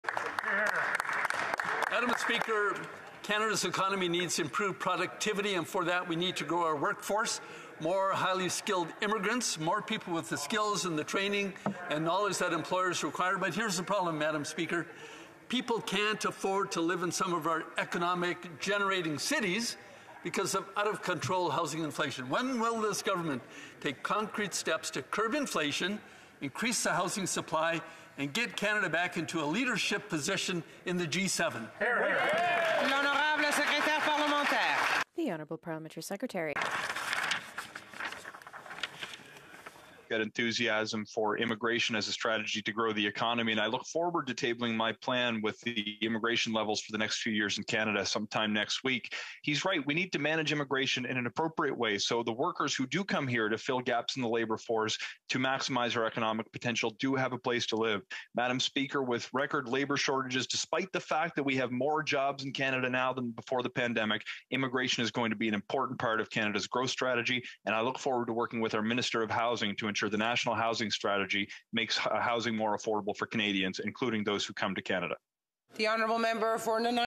Feb 11 2022 QP Housing Affordability
When will the government take concrete steps to curb inflation, increase the housing supply and get Canada back into a leadership position in the G7?